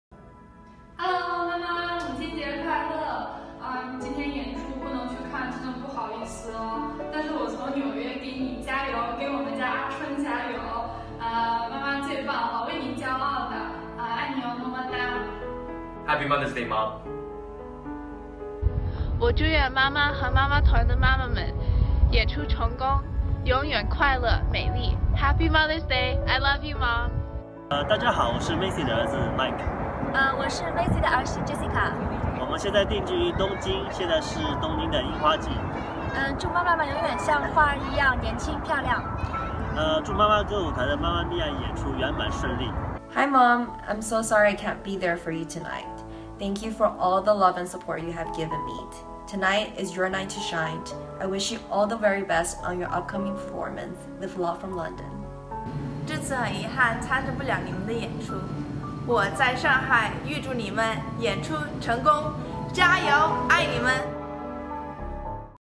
2017温哥华亲情之夜孩子们的祝福
VANCOUVER MAMA MIA～2017亲情之夜”，是一场以温哥华妈妈舞蹈团为主体演出单位的母亲节专场文艺晚会，妈妈舞蹈团团员的子女们及子女们所在艺术团体亲情参演。